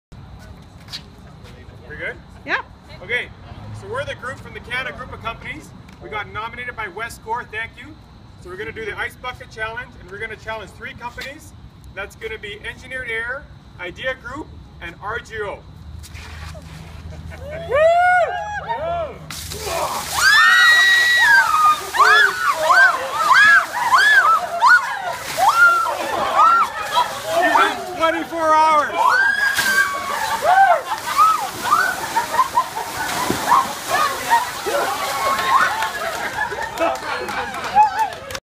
CANA ALS Challenge
The CANA Group participates in the ice Bucket Challenge in support of ALS. We nominate IDEA Group, Engineered Air and RGO. You have 24 hours!